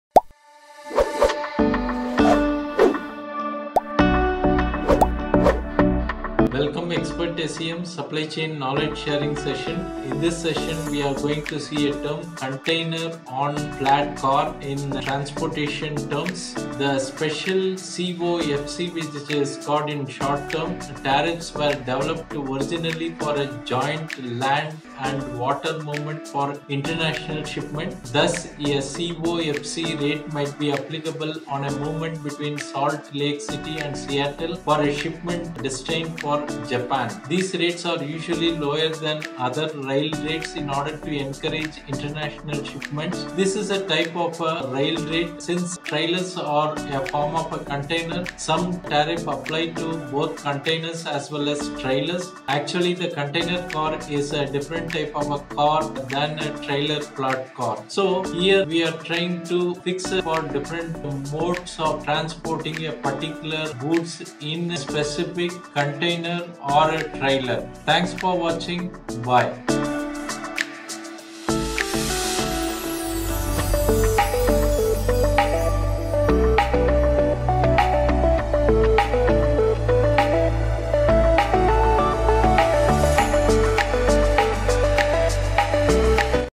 CONTAINER ON FLAT CAR | Knowledge Terms | Supply Chain || TOPIC - 105
What is Container On Flat Car in Supply Chain?
A Container on Flat Car (COFC) is a container that is transported on a rail flatcar. It can be shipped via tractor/trailer using a chassis as the wheel section.

COFCs are used for moving freight long distances and are popular for shipping goods overseas.

Containers are single-stacked or double-stacked on a flatcar, depending on the size and weight of the container.

The most common sizes for COFCs are 20- and 40-foot containers.

Containers are usually loaded on to flat cars in intermodal logistics facilities or at rail depots.

Containers are lifted onto the flatcar by a forklift or crane and secured in place with metal straps.

The doors of the container are then closed and locked.